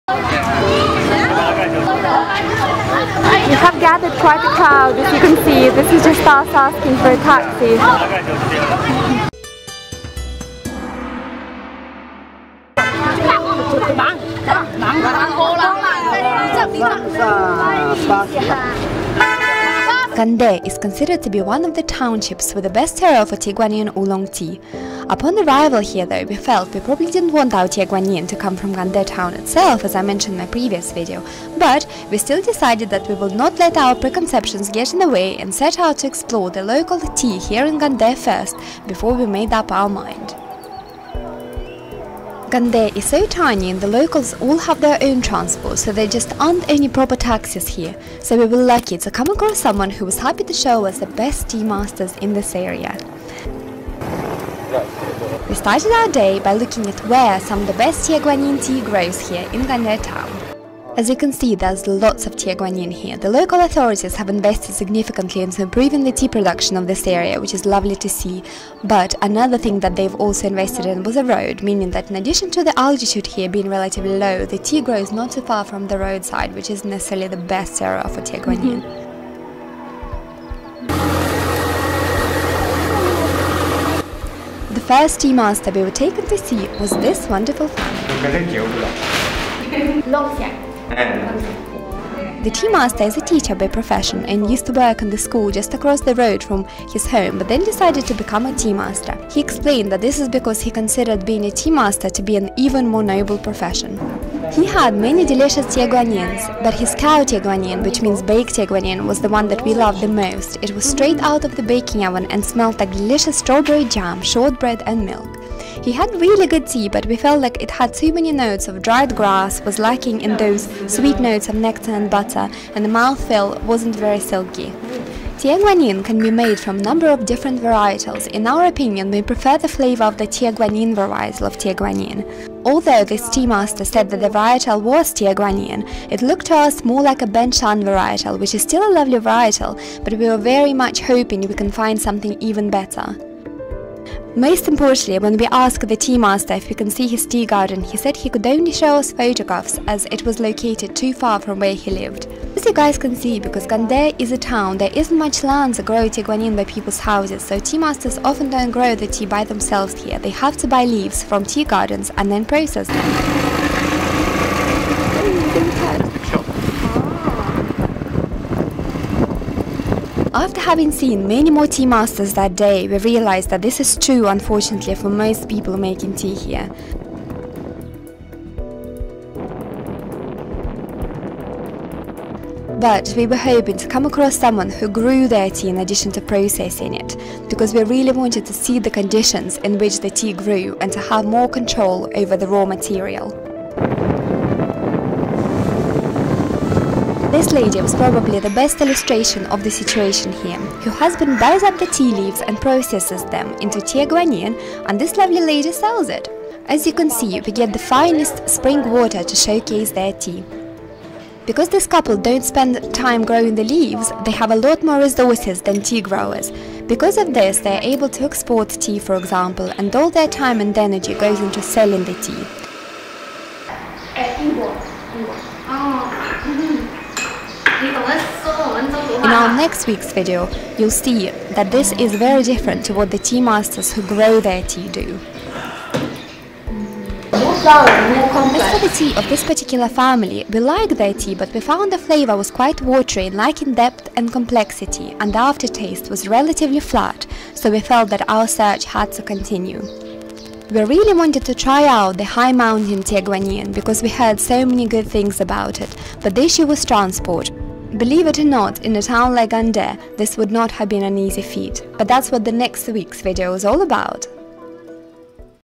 How To Find Best Tie Guan Yin Tea | Gande part 2
In this video, on our quest to find the best Tie Guan Yin oolong tea, and after having arrived in Gande - the town considered the birthplace of some of the finest Tie Guan Yin oolong, we decide to try out the tea in Gande town. In this video, we reveal what the tea is like in Gande town itself and invite you to come along and discover this with us, without the travel pains! :) Here we will also give you a little sneak peak of how we select Tie Guan Yin and on how to find the best Tie Guan Yin oolong tea - at least, according to us. :) Please join us on this super exciting adventure!

MUSIC
» A Shamaluev

Lots of love & gratitude for your support,
❤ Yulia